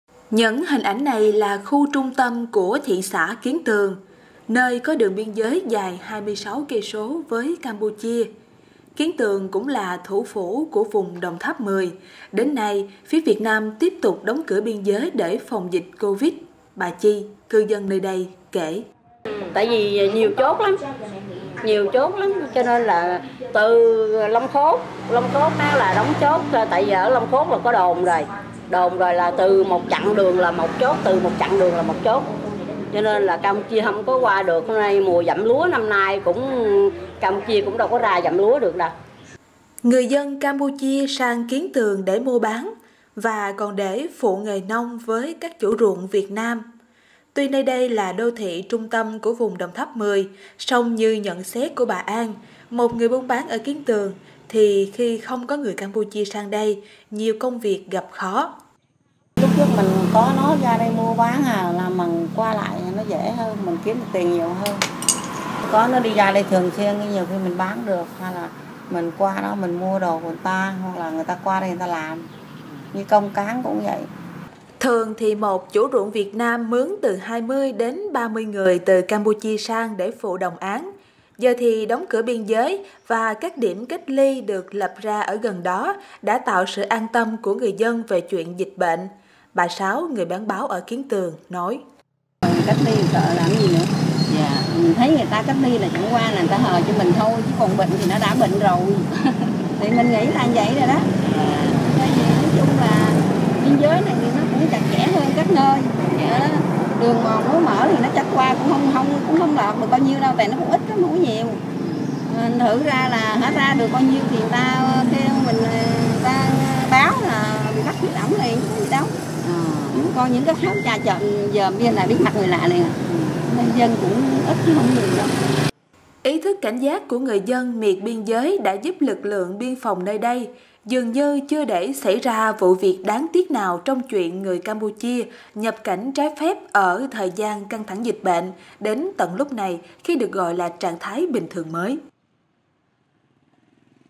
Đời sống người dân vùng biên giới trong mùa dịch Covid (VOA)
Những hình ảnh này là khu trung tâm của thị xã Kiến Tường, nơi có đường biên giới dài hai mươi sáu cây số với Campuchia. Kiến Tường cũng là thủ phủ của vùng Đồng Tháp Mười. Đến nay, phía Việt Nam tiếp tục đóng cửa biên giới để phòng dịch Covid. Bà Chi, cư dân nơi đây, kể:

“Nhiều chốt lắm, từ Long Cốt là đóng chốt, ở Long Cốt là có đồn rồi. Từ một chặng đường là một chốt, từ một chặng đường là một chốt, cho nên là Campuchia không có qua được. Mùa dặm lúa năm nay, Campuchia cũng đâu có ra dặm lúa được đâu”.

Người dân Campuchia sang Kiến Tường để mua bán, và còn để phụ nghề nông với các chủ ruộng Việt Nam. Tuy nơi đây là đô thị trung tâm của vùng Đồng Tháp Mười, song như nhận xét của bà An, một người buôn bán ở Kiến Tường, thì khi không có người Campuchia sang đây, nhiều công việc gặp khó.

“Lúc trước mình có nó ra đây mua bán, qua lại thì mình kiếm được tiền nhiều hơn. Không có nó qua, mình bán cũng không được. Có nó đi ra đây thường xuyên nhiều khi mình bán được. Mình qua đó mình mua đồ người ta, hoặc là người ta qua đây người ta làm. Như công cấy cũng vậy. Như cái mùa bình thường đó, tới gặt lúa nó ra đông lắm, thì nó rẻ, còn không mình mướn mắc”.

Thường thì một chủ ruộng Việt Nam mướn từ hai mươi đến ba mươi người từ Campuchia sang để phụ đồng áng. Giờ thì đóng cửa biên giới, và các điểm cách ly được lập ra ở gần đó đã tạo sự an tâm của người dân về chuyện dịch bệnh. Bà Sáu, người bán báo ở Kiến Tường, nói:

“Cách ly rồi sợ làm cái gì nữa. Thấy người ta cách ly là chẳng qua là người ta hờ cho mình thôi. Còn bệnh thì nó đã bệnh rồi. Nói chung là biên giới này cũng chặt chẽ hơn các nơi. Đường mà mới mở thì nó qua cũng chẳng bao nhiêu đâu, tại nó cũng ít lắm, không có nhiều. Thành thử ra là hễ qua được bao nhiêu là người ta báo, bị bắt vô trỏng liền. Còn những thứ trà trộn vào thì biết mặt người lạ liền. Nông dân cũng ít lắm, chứ không có nhiều đâu”.

Ý thức cảnh giác của người dân miệt biên giới đã giúp lực lượng biên phòng nơi đây dường như chưa để xảy ra vụ việc đáng tiếc nào trong chuyện người Campuchia nhập cảnh trái phép ở thời gian căng thẳng dịch bệnh đến tận lúc này, khi được gọi là trạng thái bình thường mới.